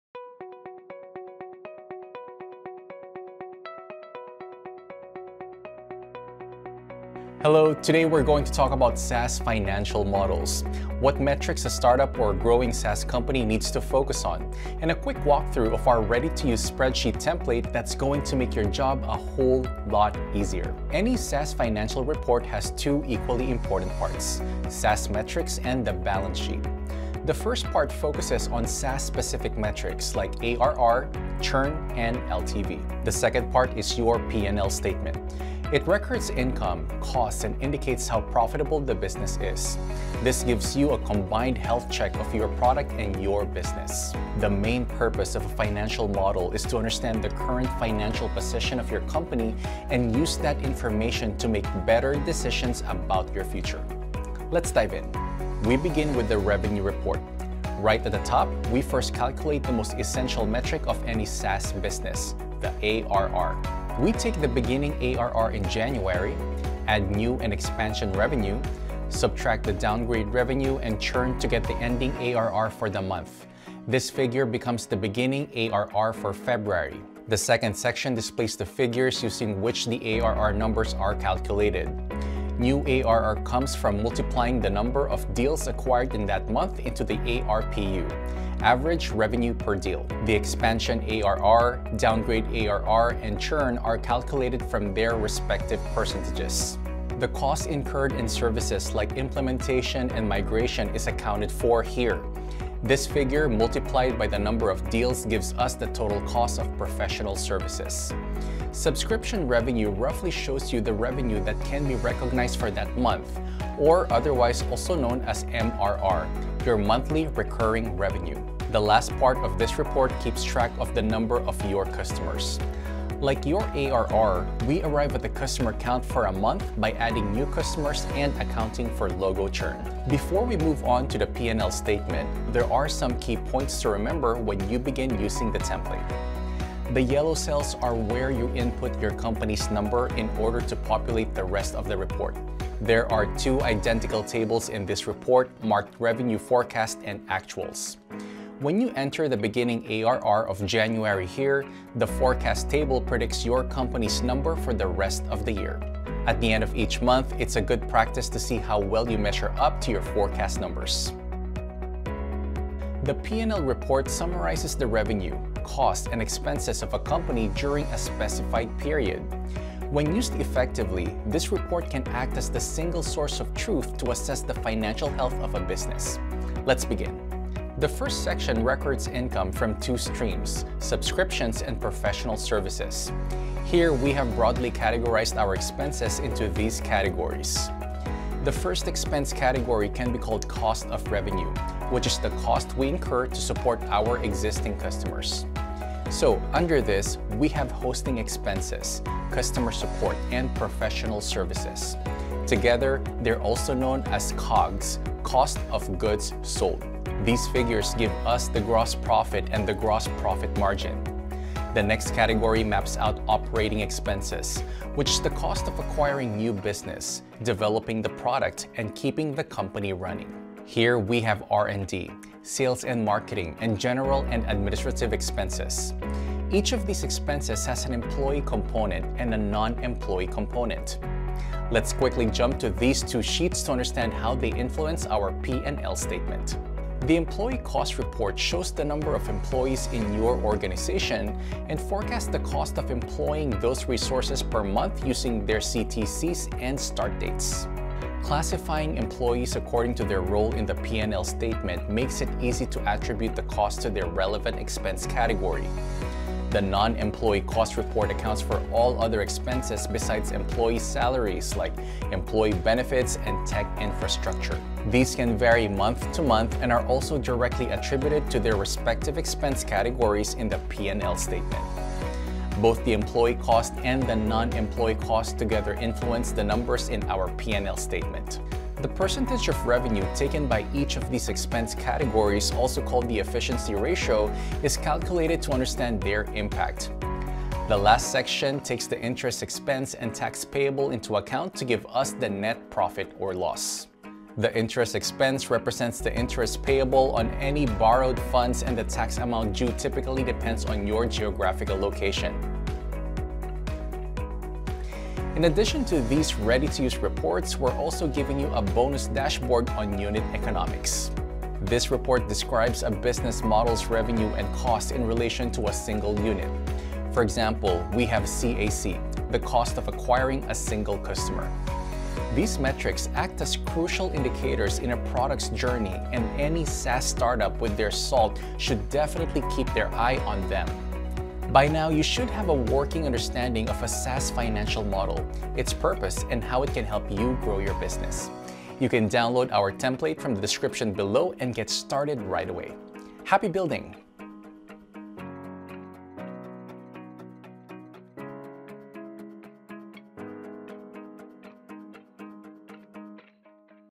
SaaS Financial Model with Template | Chargebee
SaaS financial modeling is a process of charting a summary of your SaaS's expenses and revenues. It also includes present and future revenue forecasts and important KPIs that a finance leader must monitor.
Our plug-and-play Excel template for the SaaS financial model is fairly easy to use. This video provides a complete walkthrough of our template.